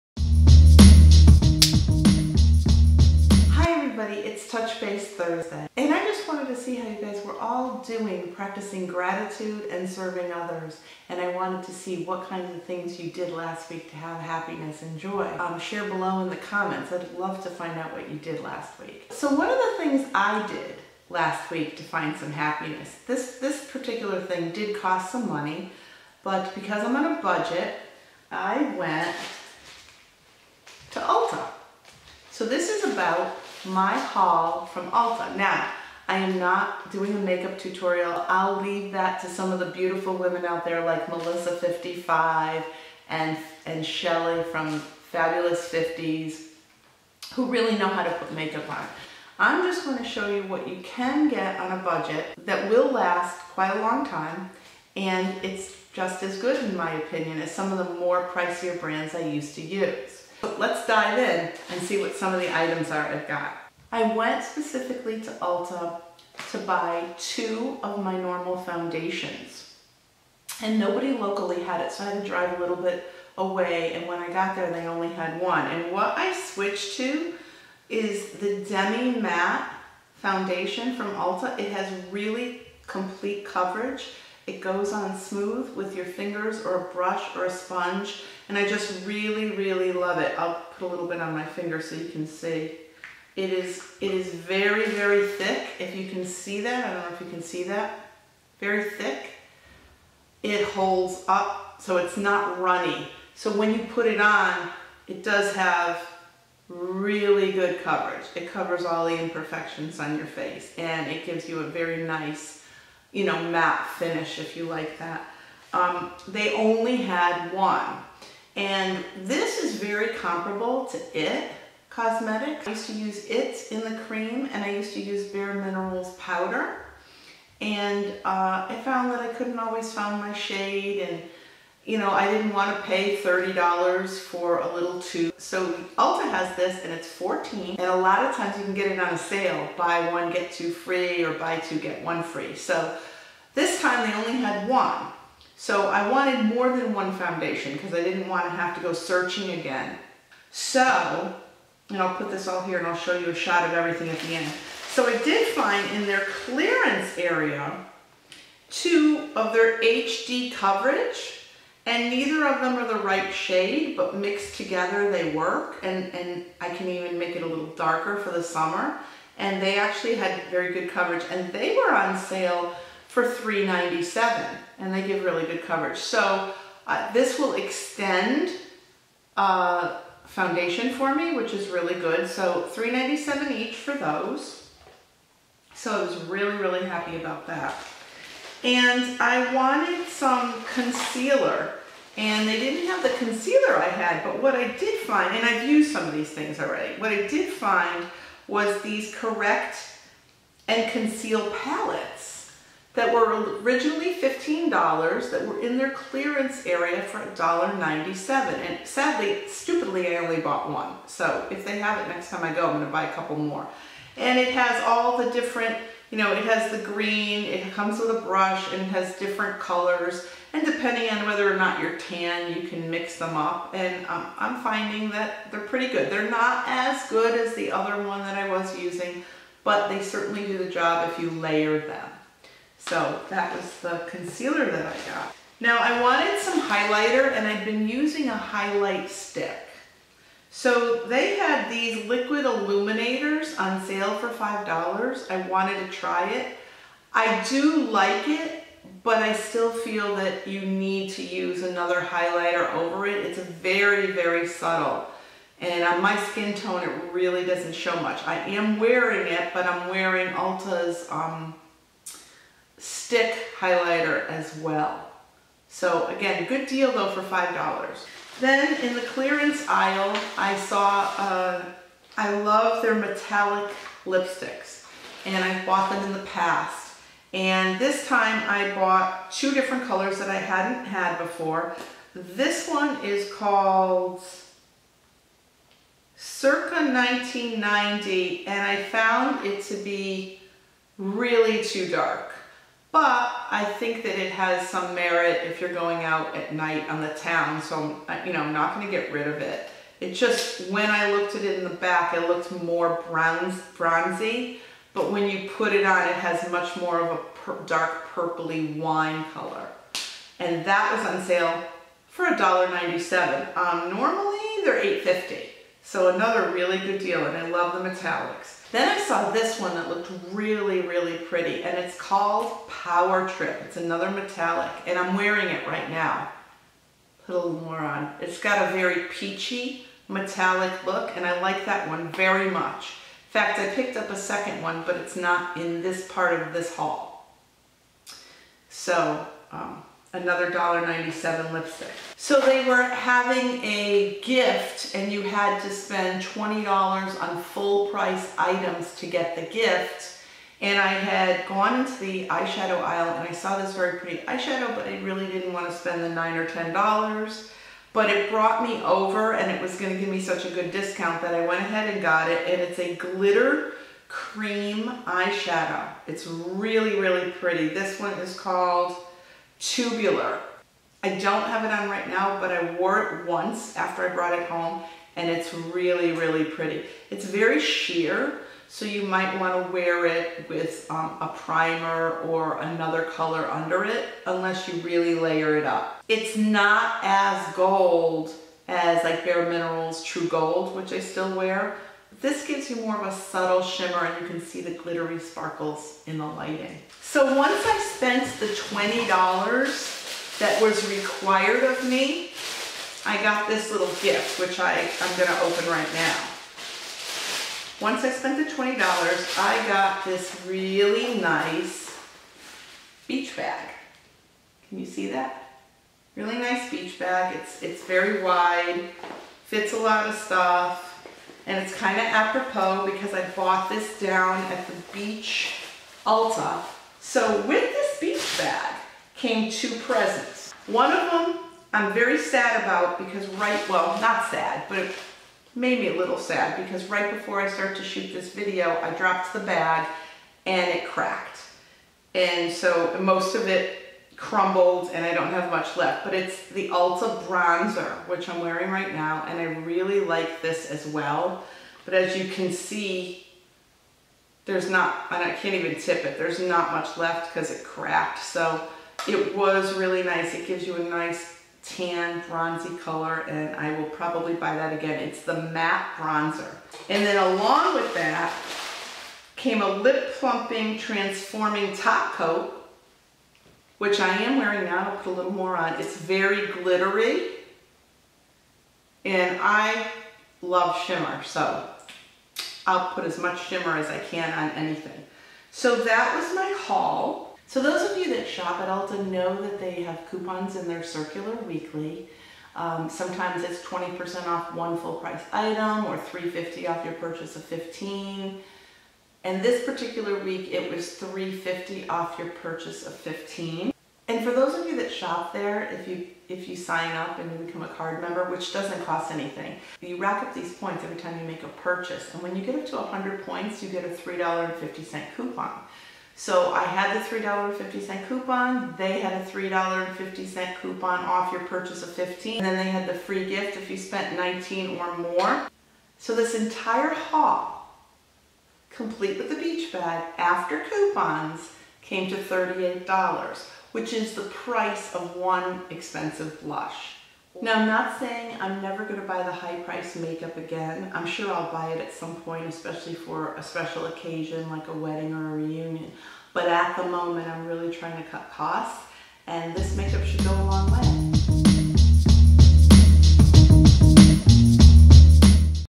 Ulta Clearance Haul June 2019
Golden Hour presents Touch Base Thursday, which is a time to touch base and see how you are doing with what you learned in the previous Golden Hour about finding joy. Today's episode also shares an Ulta Clearance Haul with discounted makeup. I show you all of the everyday makeup that I got for a very affordable price.